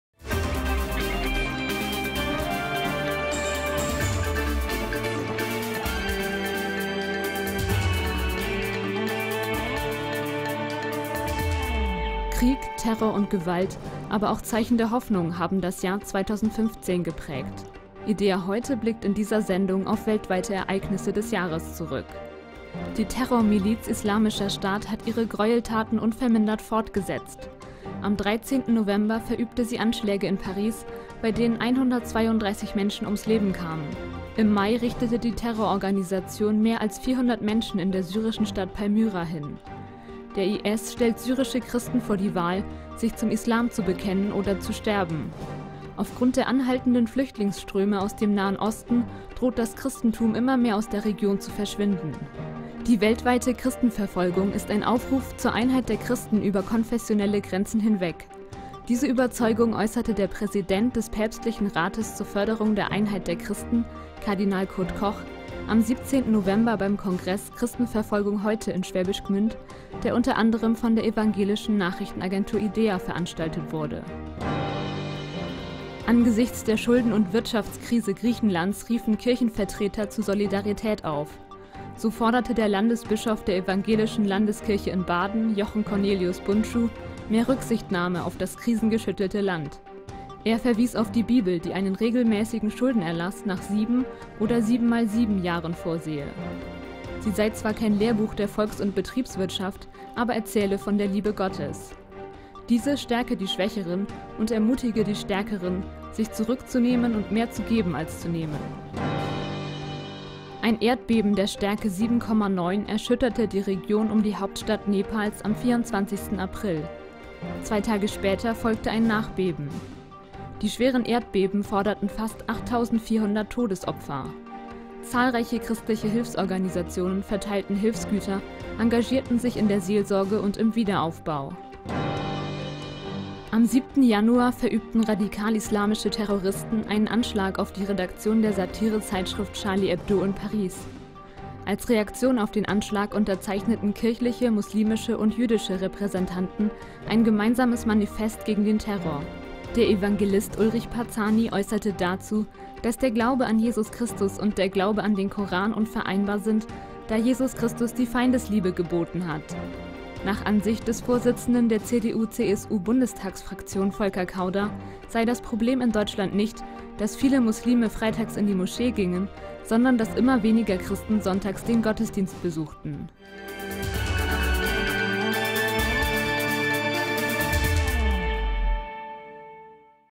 ideaHeute 2015 12 28 Jahresrückblick 2015 international
Die Anschläge in Frankreich und die Verfolgung von Christen durch den Islamischen Staat bewegten die Welt. idea blickt auf die Ereignisse des Jahres zurück.